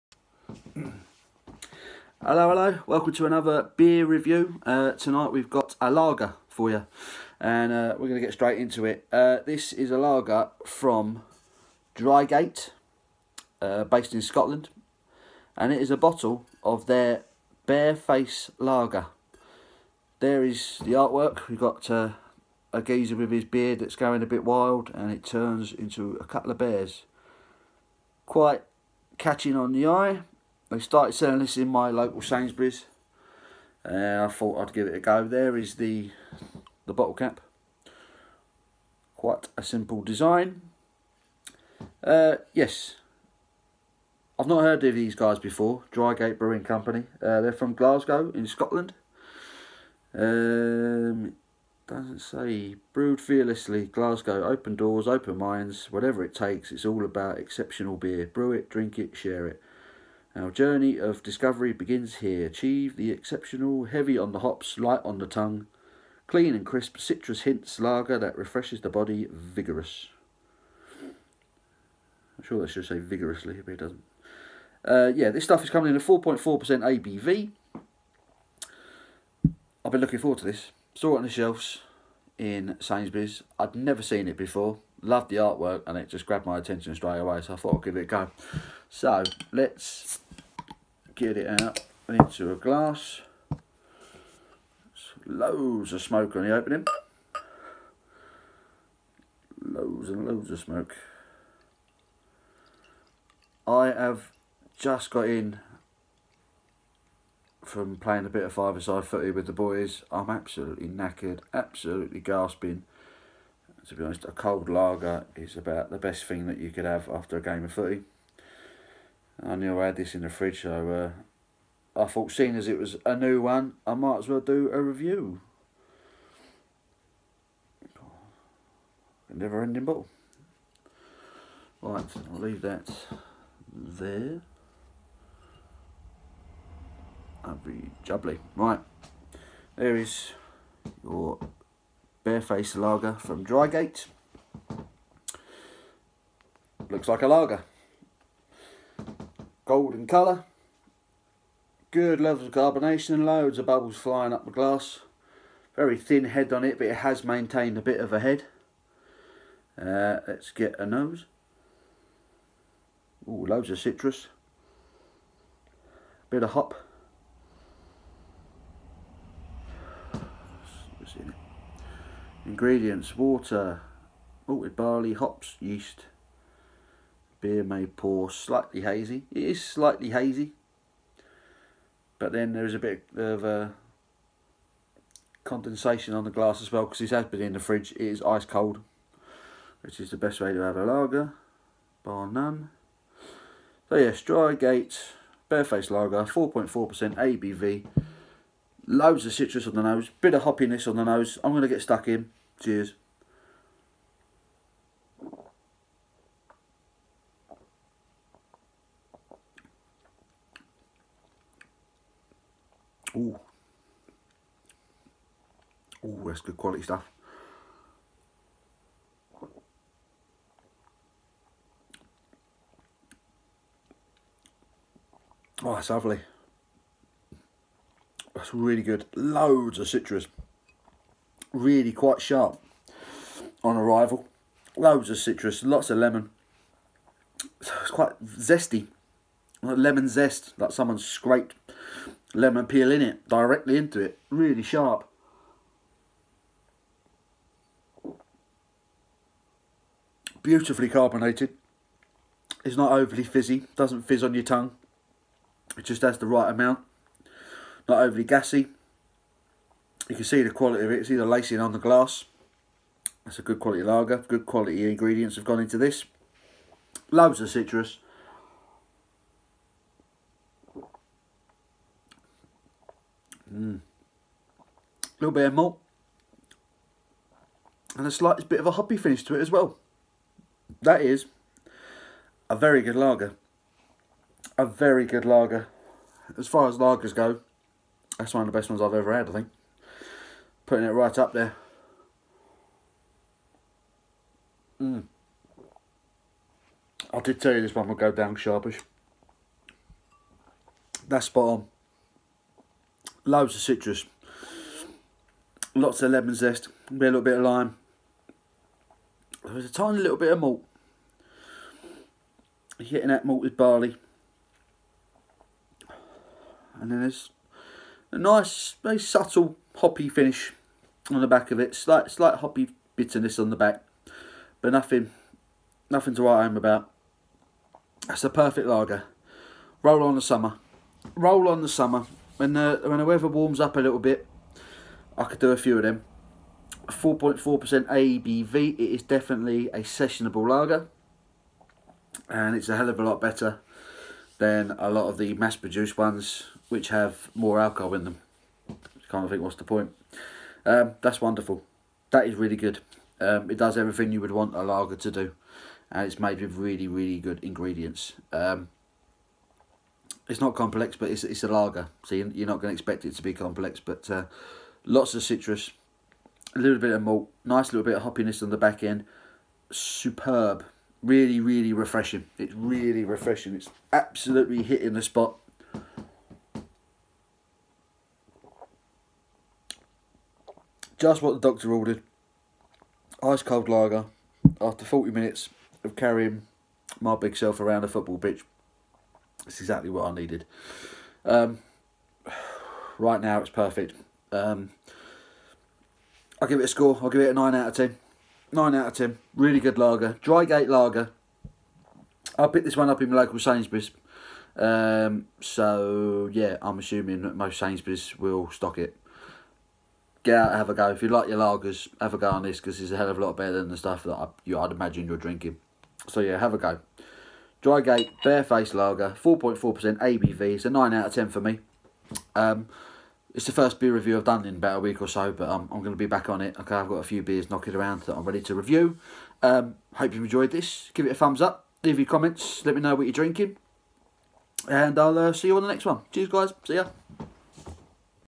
Beer Review #22: Drygate - Bearface Lager
Beer description, opening, smell, taste & personal opinion